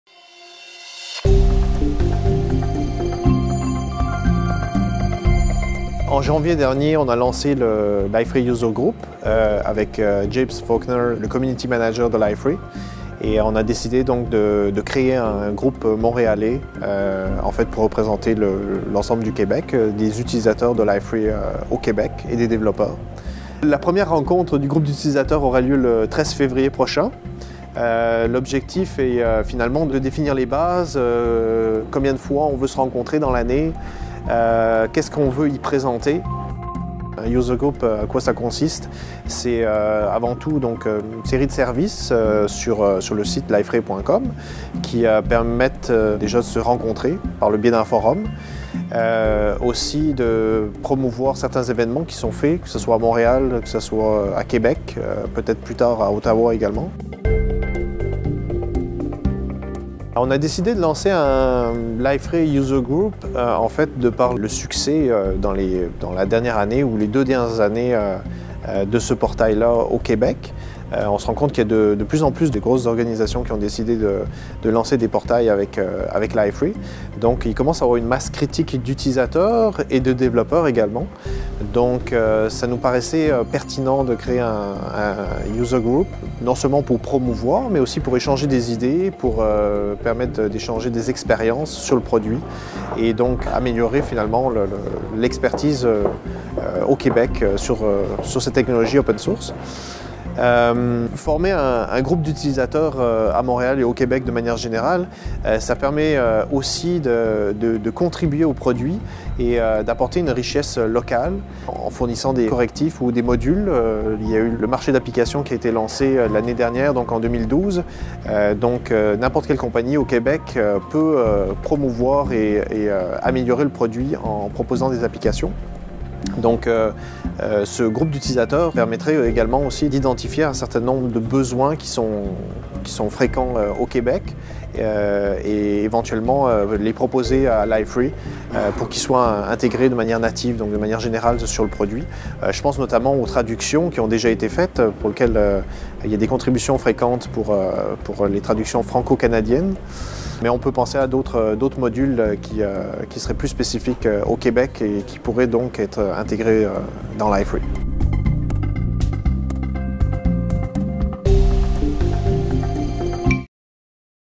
Savoir-faire Linux lance le Liferay Montreal User Group
Montréal, le 4 février 2013 - Savoir-faire Linux, un leader de l'intégration des logiciels libres et du soutien des technologies ouvertes au Québec et au Canada, lance le 13 février le premier groupe d'utilisateurs Liferay au Canada La technologie de portails Web Liferay atteint en effet une masse critique au Québec. Les explications de Sven Werlen, vice-président, Solutions d'entreprise chez Savoir-faire Linux. Plus de détails